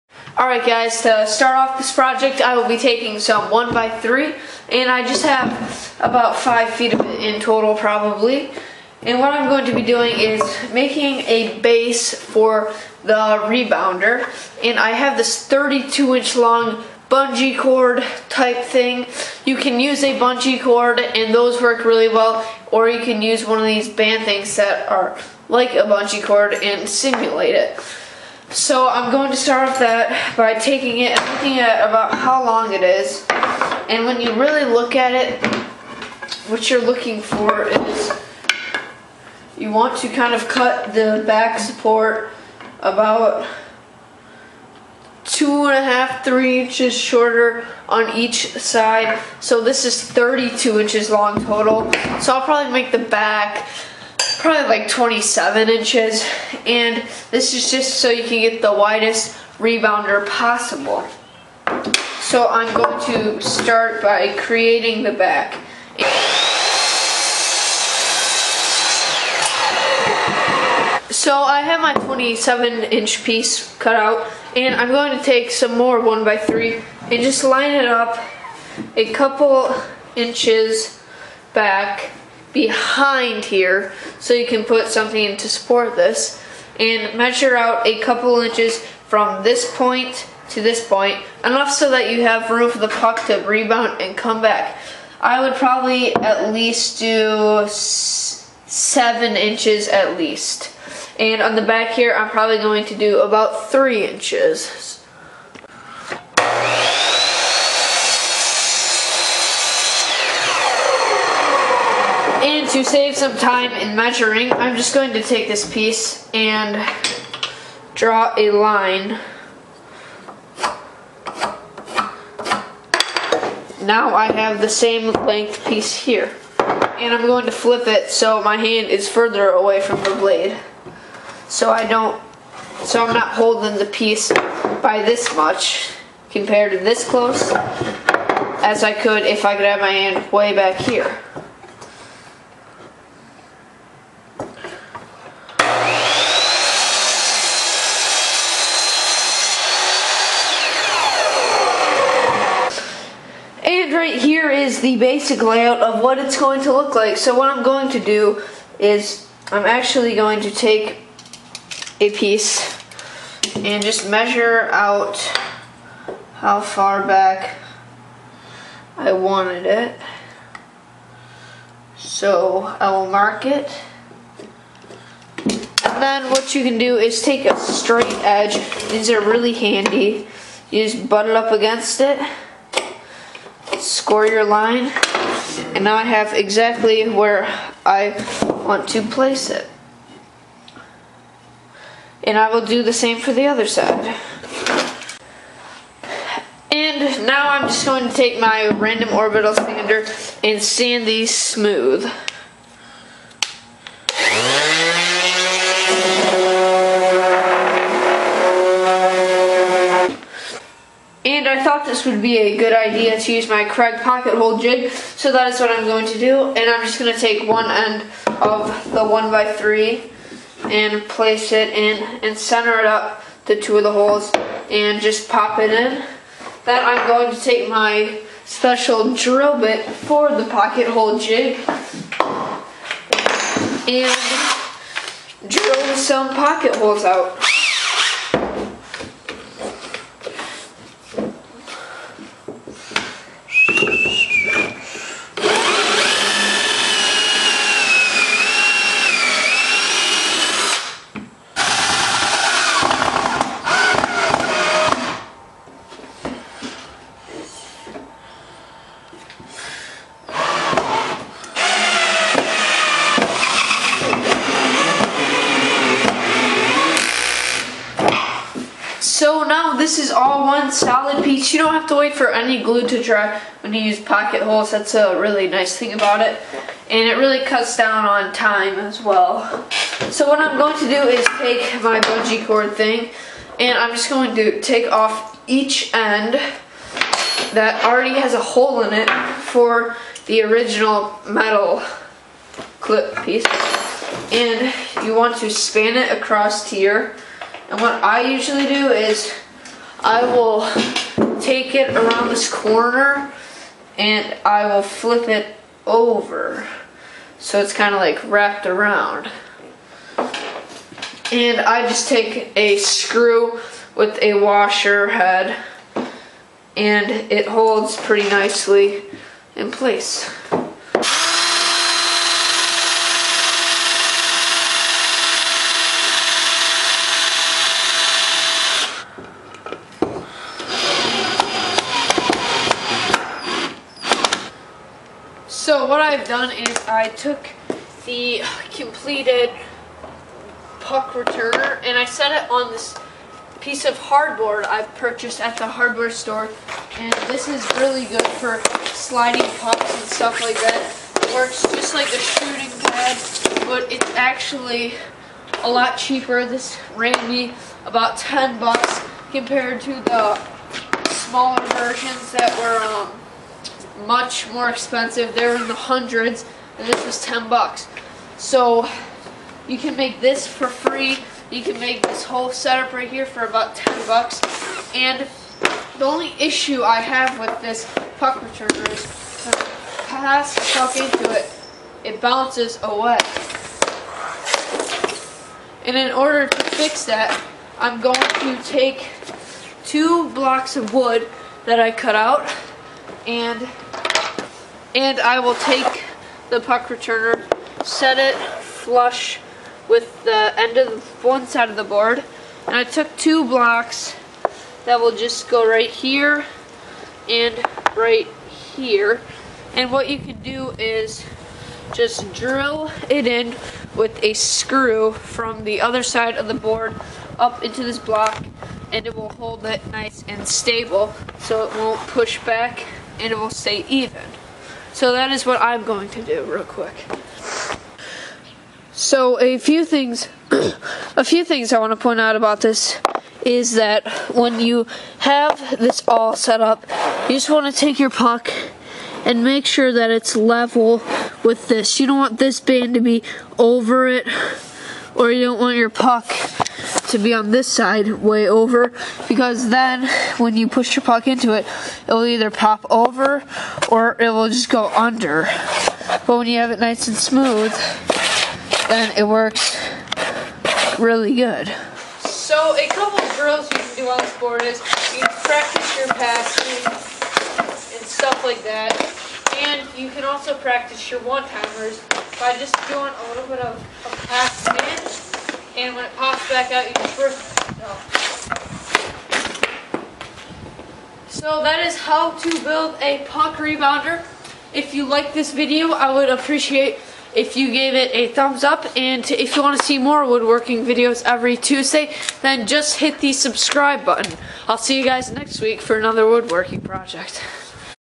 How to Make a Hockey Puck Rebounder: Less than 6 Bucks!
For More Information about this Project (Plans, Cutting Templates, SketchUp Models, etc.)